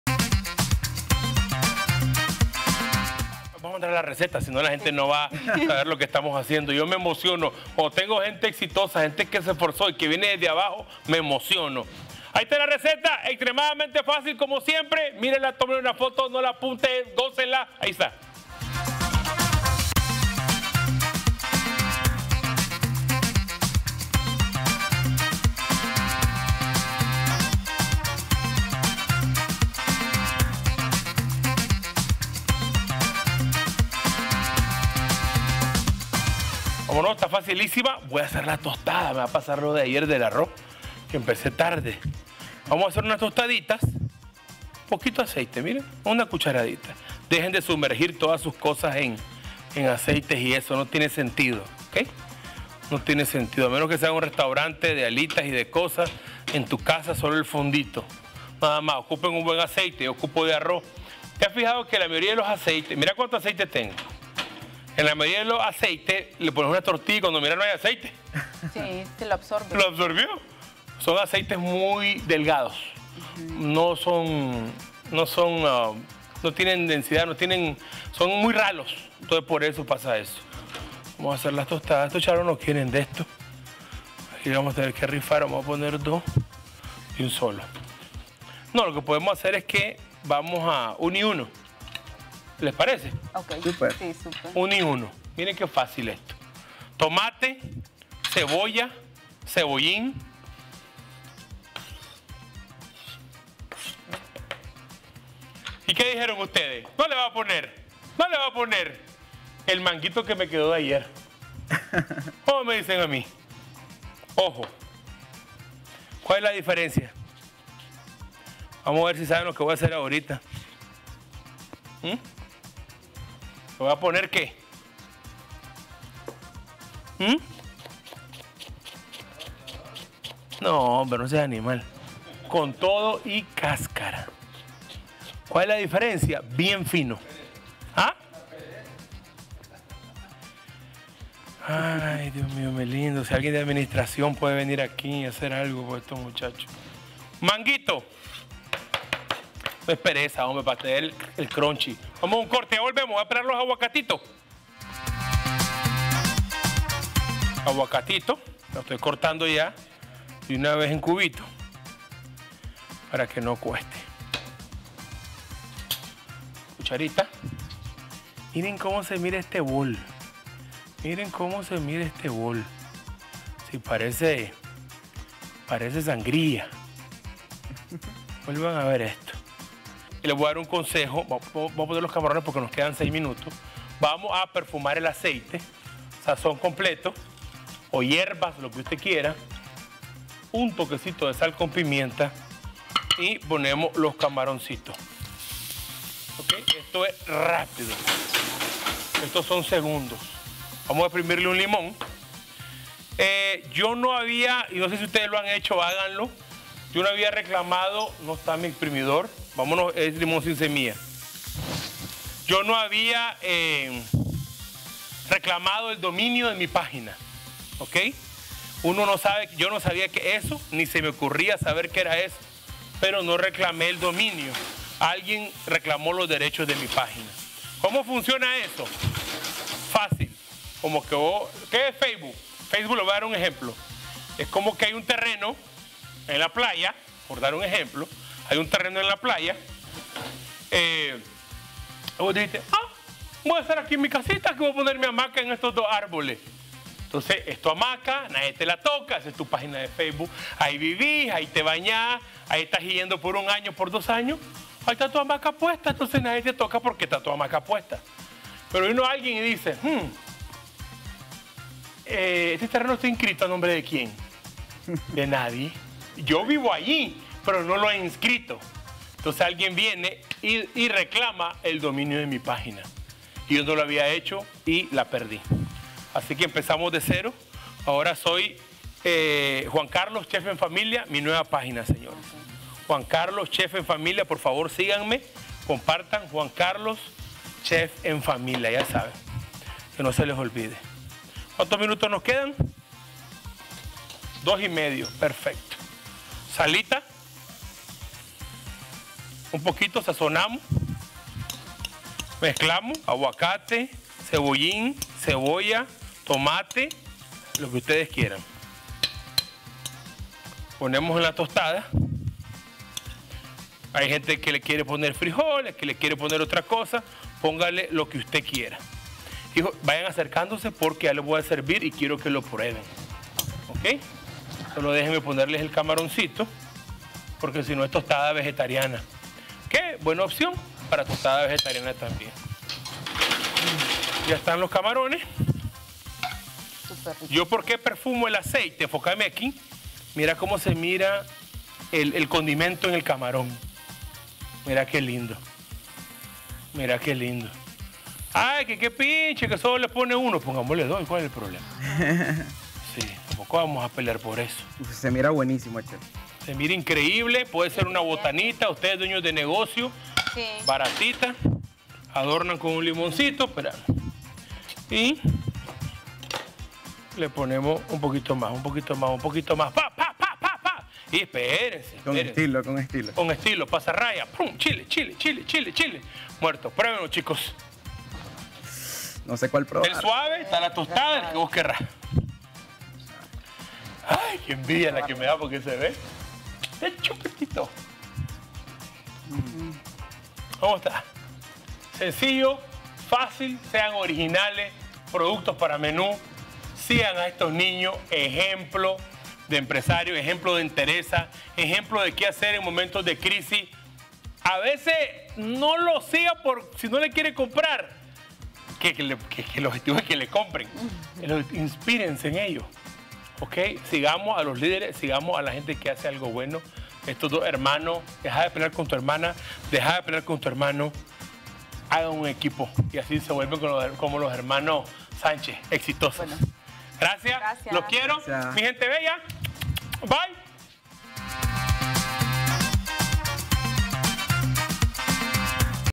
Tostadas de camarón || NOCHES CON SABOR
La receta de tostadas de camarón no se escapa del delicioso sabor de este crustáceo.